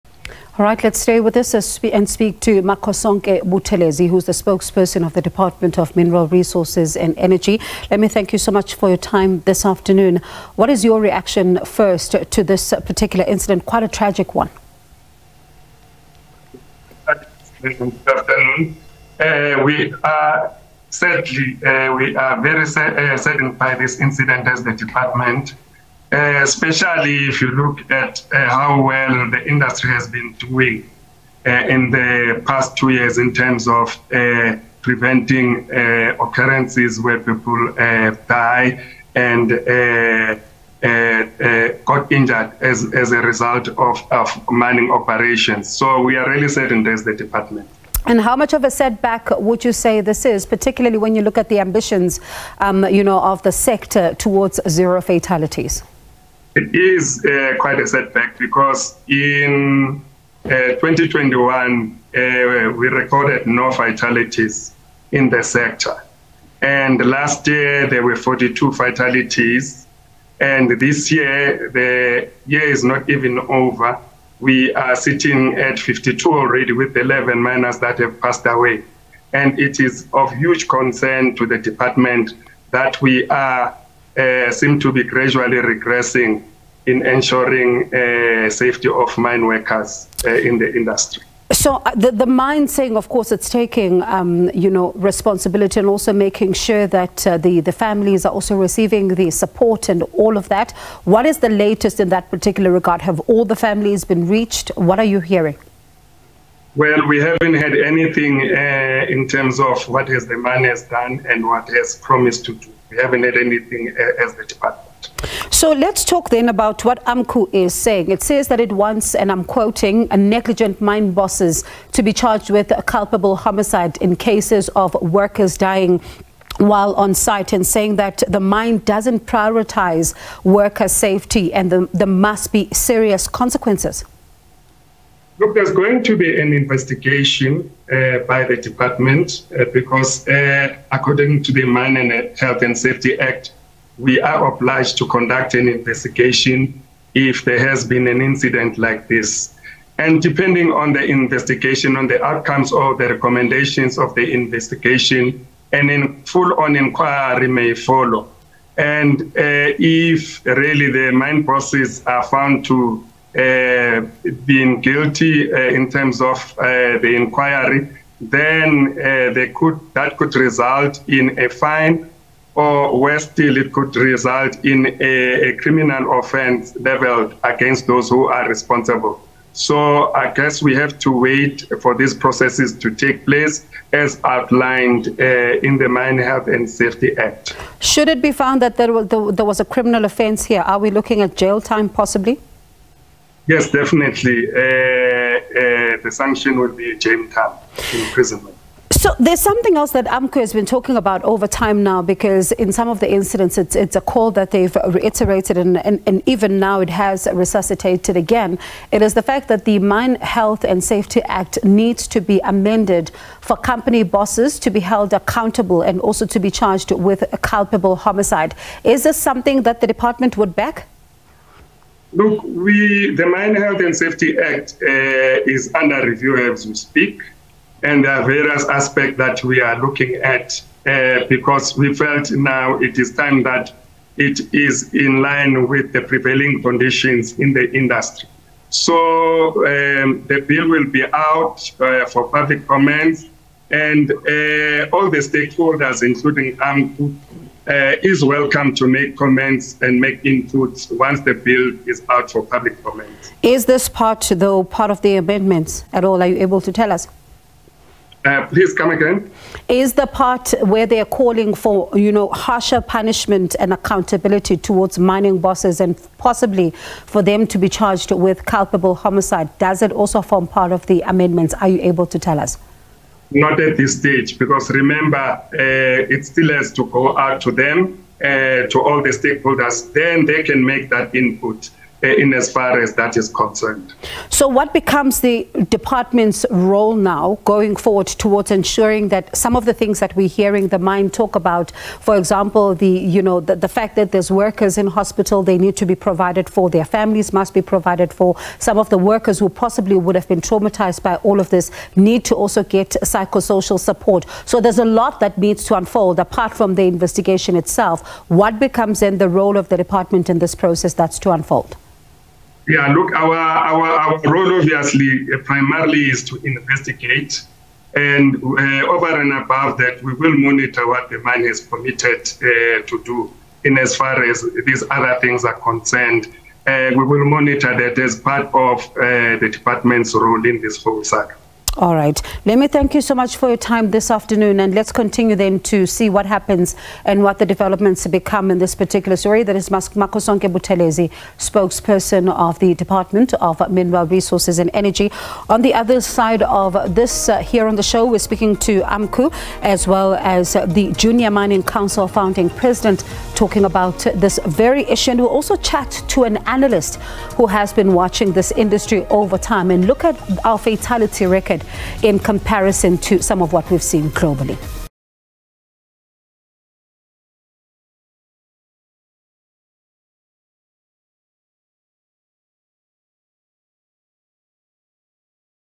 11 killed, 75 injured in an underground accident at Impala Platinum Mine: Makhosonke Buthelezi
Eleven people have died and 75 others have been injured in an underground accident at an Implats mining facility near Rustenburg.
Implats says the accident linked to the winder rope happened at the mine's 11 Shaft operation on Monday.
The rope is connected to the personnel conveyance, which hoists employees up and down the shaft.
The conveyance comprises three levels, each with a capacity to carry 35 personnel
Makhosonke Buthelezi, Spokesperson Department of Mineral Resources and Energy